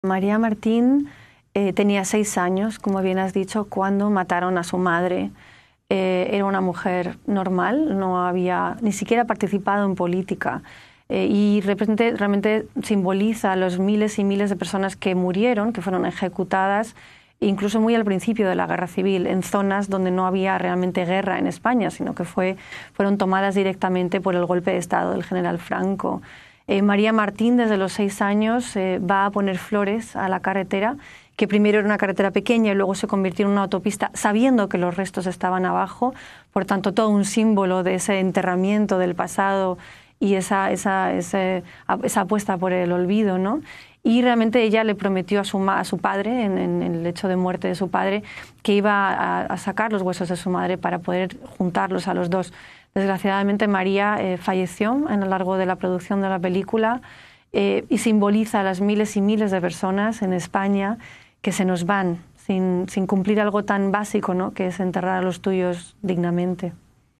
“El silencio de otros”: historias de las víctimas del fascismo en España
Hablamos con la cineasta Almudena Carracedo quien escribió, produjo y dirigió “El silencio de otros”, un nuevo y notable documental quiere recordar lo ocurrido en el franquismo en España a las nuevas generaciones, para que ese pasado no se repita. En este segmento, Carracedo cuenta una de las historias incluidas en la película, la de María Martín.